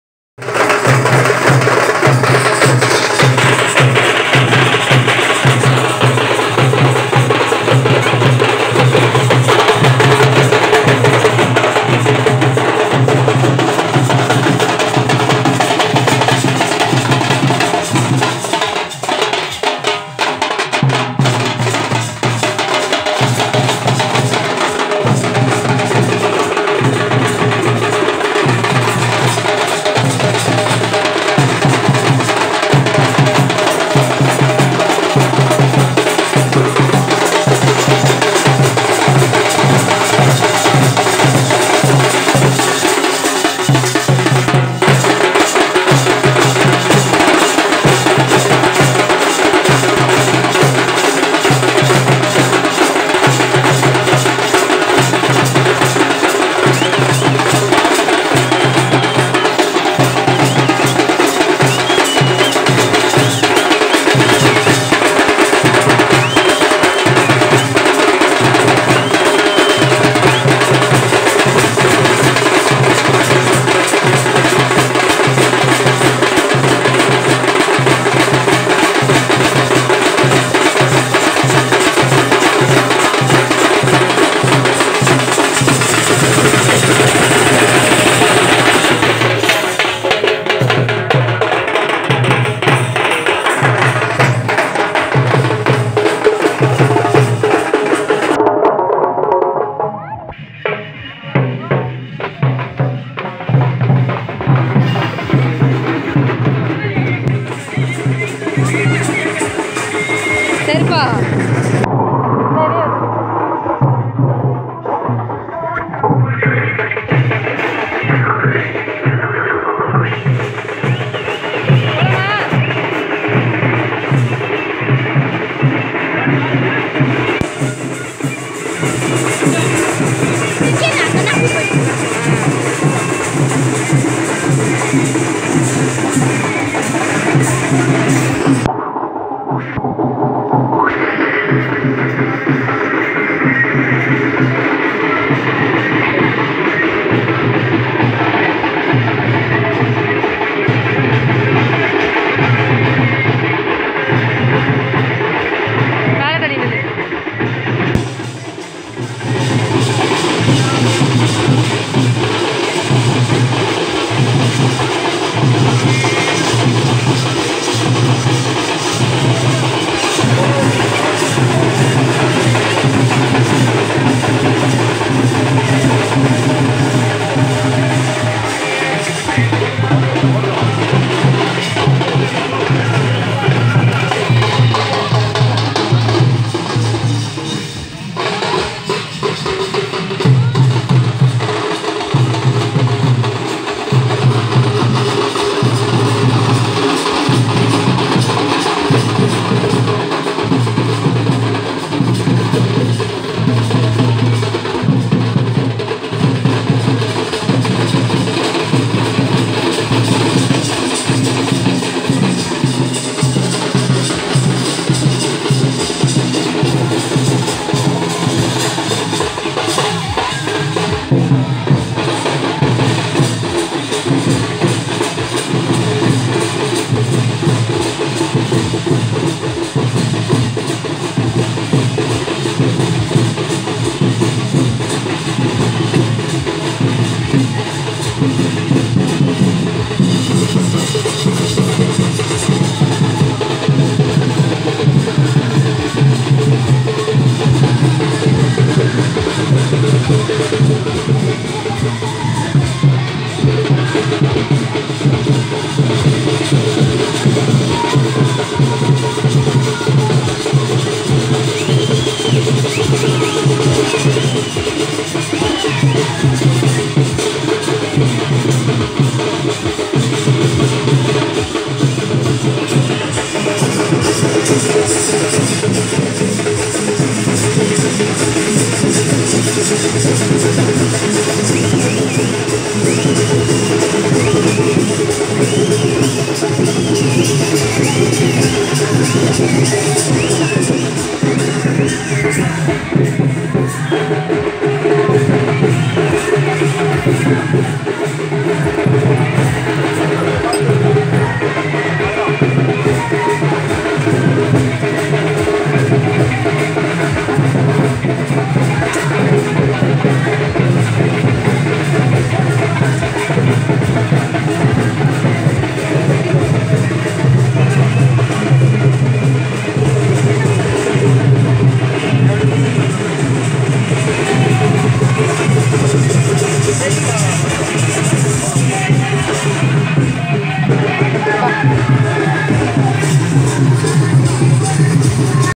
Temple festival At My Native place manampalli estate Valparai
Alagu kuthum vizha 2018